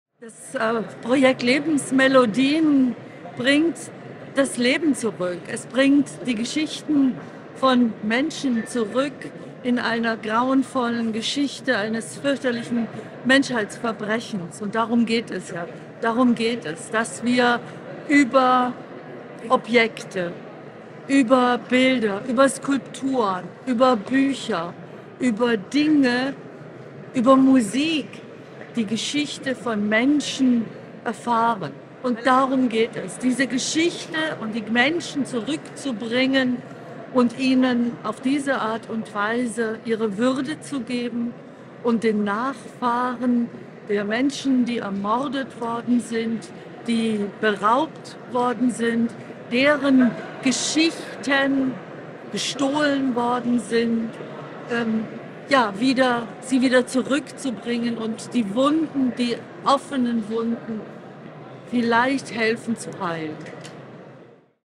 Kulturstaatsministerin Claudia Roth zu Lebensmelodien
Claudia Roth, Beauftragte der Bundesregierung für Kultur und Medien, sprach im Anschluss an ein Lebensmelodien-Konzert über die Bedeutung der Arbeit des Projektes “Lebensmelodien”. Dabei betonte sie unter anderem den Wert von Musik in der Erinnerung an die Shoah, um die Lebensgeschichten und Werke jüdischer Menschen sichtbar zu machen.

Im Vordergrund stehen bei den “Lebensmelodien” jüdische Musik und die Schicksale der damit verbundenen Menschen. Es sind musikalische Stücke, die von jüdischen Menschen in der Zeit von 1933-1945 geschaffen, gespielt und gesungen wurden und nun wieder aufgeführt werden.

Das Konzert war Teil der Auftaktveranstaltung des Erinnerungsprojektes “Kunst, Raub, Rückgabe” im Bode-Museum Berlin, welches von der Stiftung Preußischer Kulturbesitz und den Bayerischen Staatsgemäldegalerien in Kooperation mit dem RBB und BR initiiert wurde.